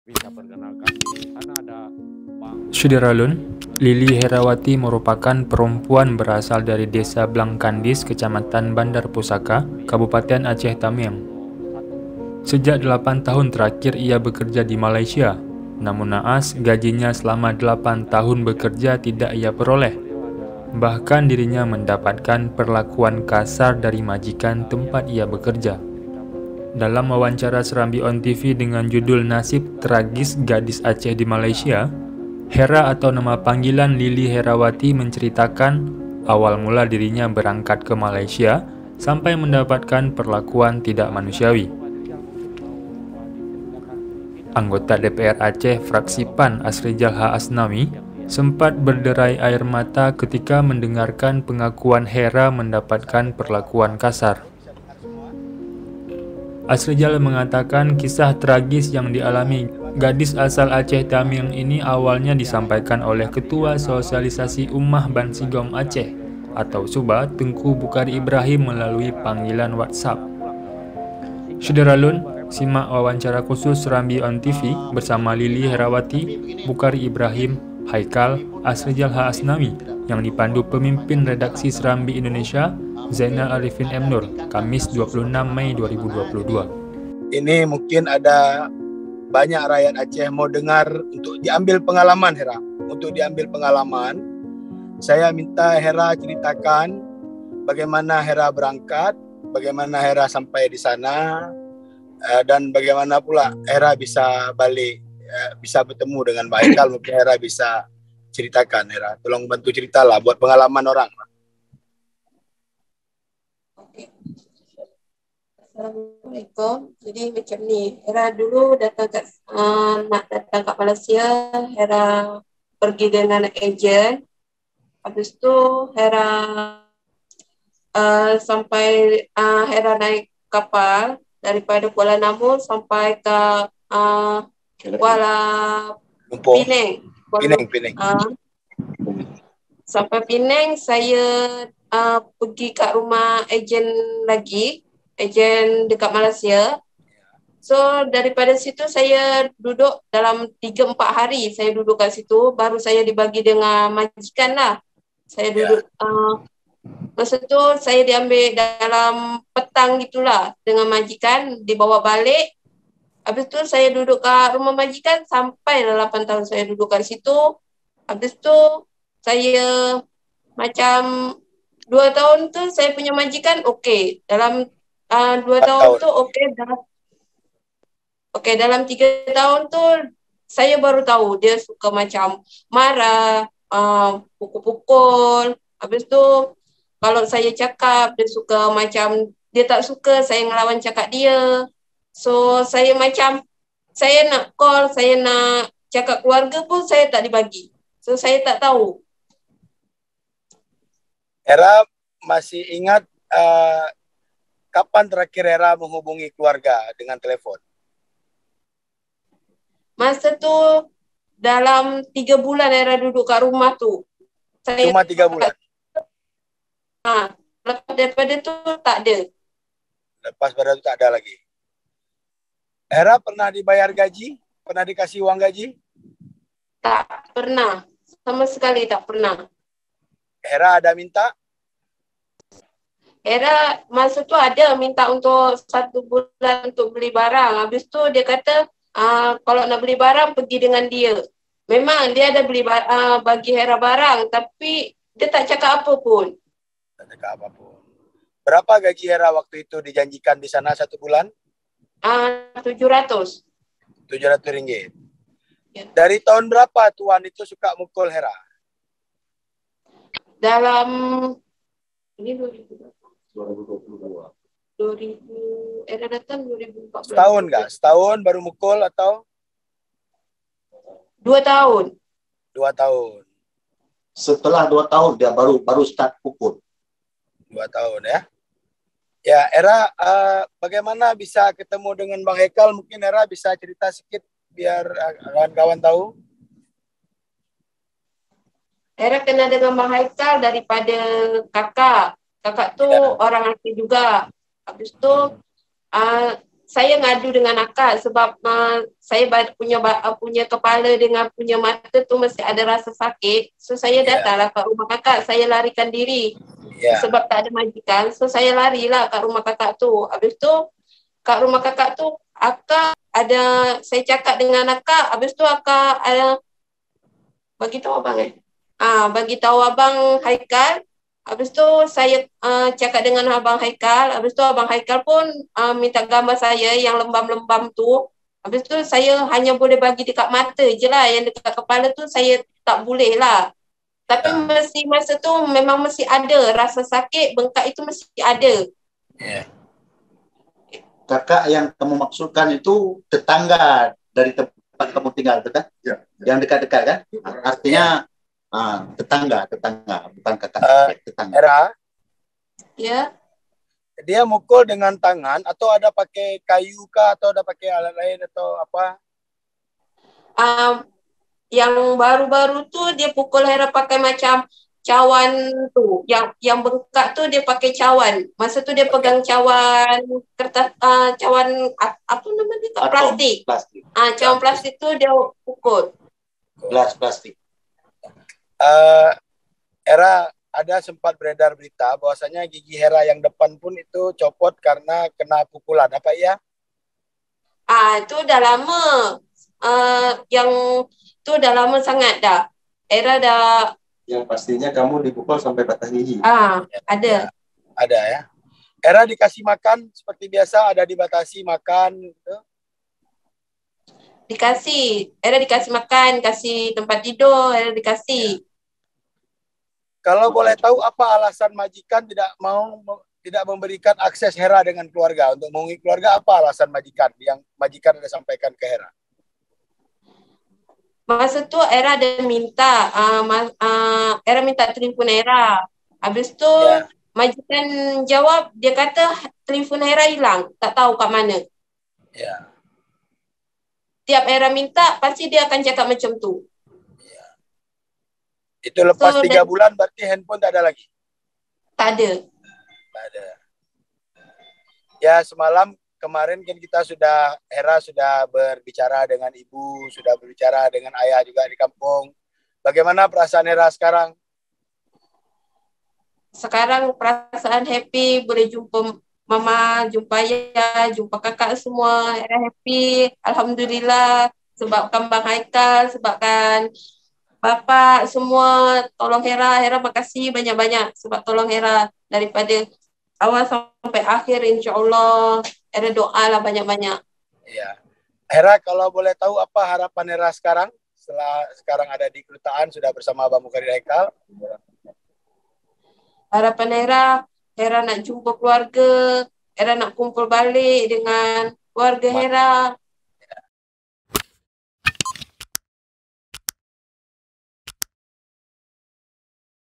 Pengakuan Lili Herawati Gadis Asal Aceh di Malaysia, Kerja 8 Tahun Tidak Dibayar sampai Patah Gigi
SERAMBINEWS.COM - Lili Herawati merupakan perempuan berasal dari Desa Blang Kandis, Kecamatan Bandar Pusaka, Kabupaten Aceh Tamiang.

Sejak delapan tahun terakhir, ia bekerja di Malaysia namun naas gajinya selama delapan tahun bekerja tidak ia peroleh, bahkan dirinya mendapatkan perlakuan kasar dari majikan tempat ia bekerja.

Dalam wawancara Serambi on TV dengan judul "Nasib Tragis Gadis Aceh di Malaysia" Hera atau nama panggilan Lili Herawati menceritakan awal mula dirinya berangkat ke Malaysia sampai mendapatkan perlakuan tidak manusiawi.

Anggota DPR Aceh Fraksi PAN Asrizal H Asnawi sempat berderai air mata ketika mendengarkan pengakuan Hera mendapatkan perlakuan kasar.

Asrizal mengatakan, kisah tragis yang dialami gadis asal Aceh Tamiang ini awalnya disampaikan oleh Ketua Sosialisasi Ummat Bansigom Acheh (SUBA), Tgk Bukhari Ibrahim, melalui panggilan WhatsApp

Syedara lon, simak wawancara khusus Serambi on TV bersama Lili Herawati, Bukhari Ibrahim, Haikal, Asrizal H Asnawi yang dipandu Pemimpin Redaksi Serambi Indonesia Zainal Arifin M Nur, Kamis (26/5/2022).